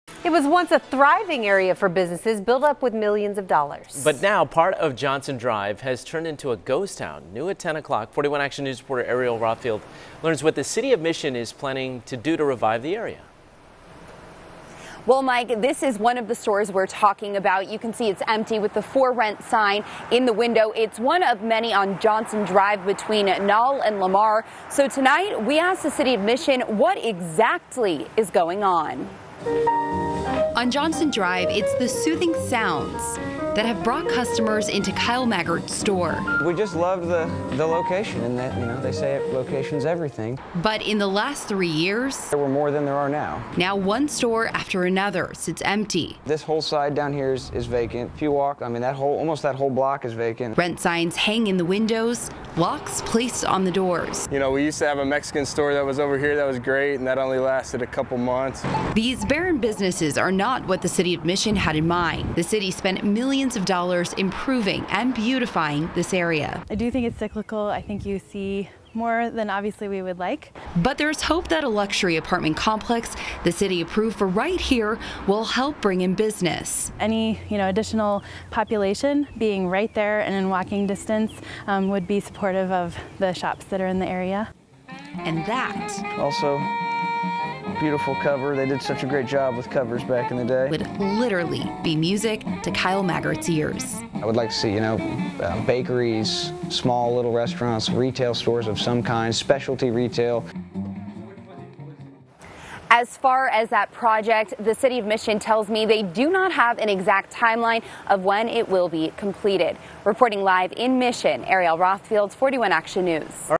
Mission battling empty storefronts on Johnson Dr.
If you drive along Johnson Drive between Nall and Lamar, you will see one storefront after another sitting empty.